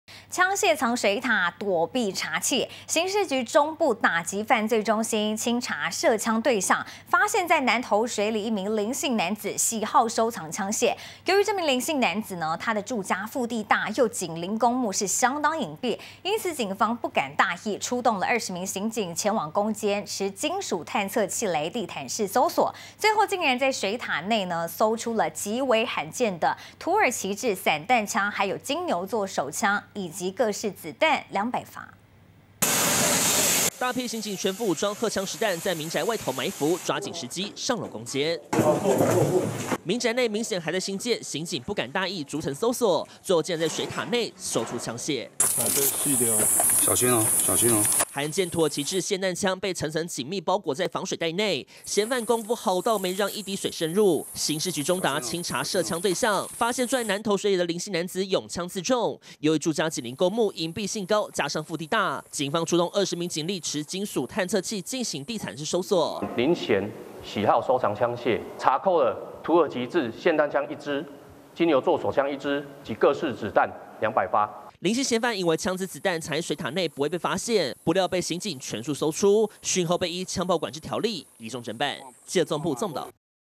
藝品商藏2槍200發子彈 20警荷槍實彈攻堅
槍械藏水塔，躲避查緝! 刑事局中部打擊犯罪中心，清查涉槍對象，發現南投水里一名林姓男子喜好收藏槍械，由於林姓男子住家腹地大，有緊鄰公墓，相當隱蔽，警方不敢大意，出動20名刑警前往攻堅，持金屬探測器地毯式搜索，最後竟然在水塔內，搜出極為罕見的土耳其製霰彈槍、金牛座手槍，以及各式子彈200發。